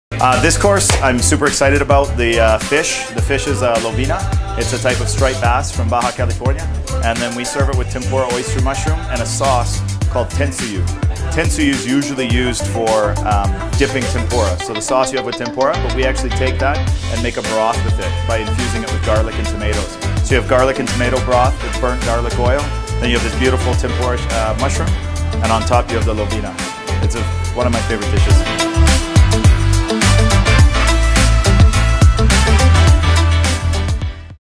Lobina en PUEBLA ➡️ tempura, tentsuyu de tomate, scallions por Darren mclean
🔥🔥Some days back! En 📍intro restaurant de puebla 🐟 lobina a la plancha ➡️ 🍄 tempura, tentsuyu de tomate, scallions 🔪 por el chef Darren Maclean thanks again Chef Angel Vázquez  🙏🏼🔥🔥